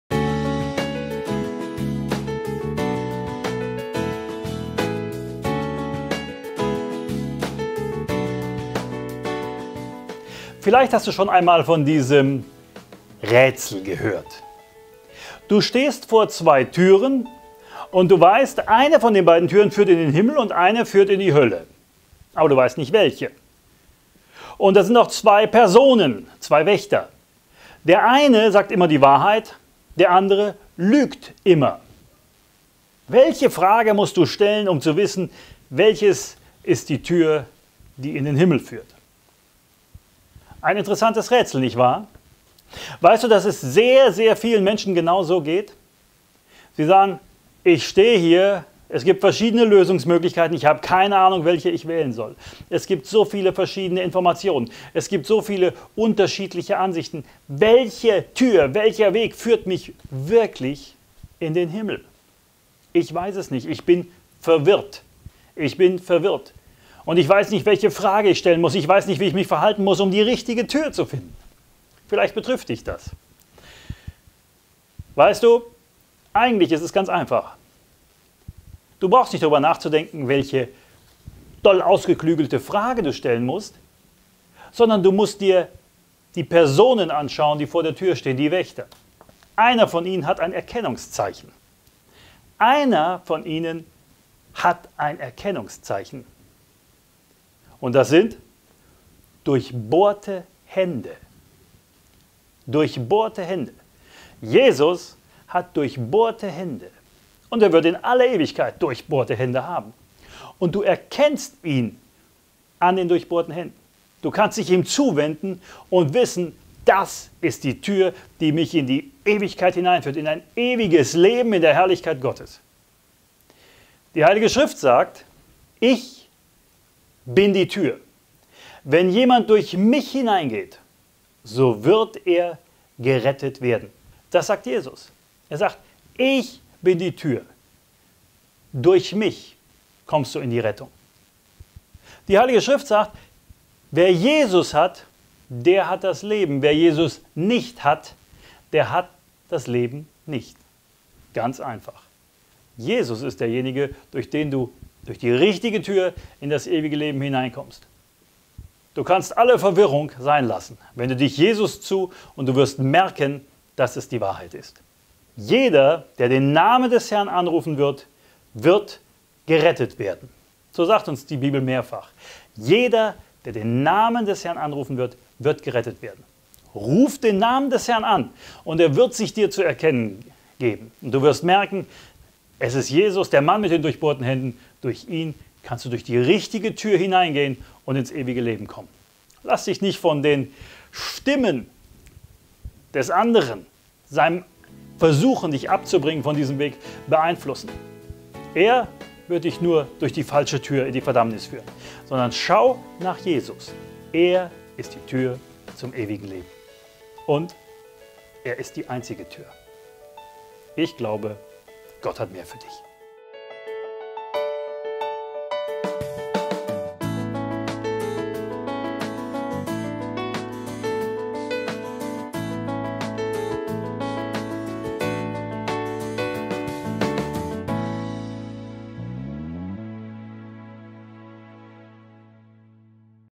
Tür zum Himmel – Tür zur Hölle
Kennst du das Rätsel: Du stehst vor zwei Türen. Eine führt zum Himmel, die andere zur Hölle. Du weißt aber nicht, welche. Vor jeder Tür steht ein Wächter. Der eine sagt immer die Wahrheit, der andere lügt immer. Welche Frage musst du stellen, um zu wissen, welche Tür dich in den Himmel bringt? So geht es vielen Menschen. Es gibt so viele Informationen und unterschiedliche Antworten, wenn es um das Leben nach dem Tod geht. Was ist Lüge, was ist Wahrheit?

Sie sind voller Verwirrung bei der Frage, was nach dem Tod kommt. Lebe ich richtig? Erlange ich das ewige Leben?
Doch es gibt eine Hilfe. Derjenige, der immer die Wahrheit sagt, trägt ein Erkennungszeichen …

Jesus sprach nun wieder zu ihnen: Wahrlich, wahrlich, ich sage euch: Ich bin die Tür der Schafe.
Johannes 10,7

Ich bin die Tür; wenn jemand durch mich hineingeht, so wird er gerettet werden
Johannes 10,9

Siehe, ich stehe an der Tür und klopfe an; wenn jemand meine Stimme hört und die Tür öffnet, zu dem werde ich hineingehen und mit ihm essen und er mit mir.
Offenbarung 3,20

Denn jeder, der den Namen des Herrn anrufen wird, wird gerettet werden.
Römer 10,13

Und es wird geschehen: Jeder, der den Namen des Herrn anrufen wird, wird gerettet werden.
Apostelgeschichte 2,21

Wer den Sohn hat, hat das Leben; wer den Sohn Gottes nicht hat, hat das Leben nicht.
1.Johannes 5,12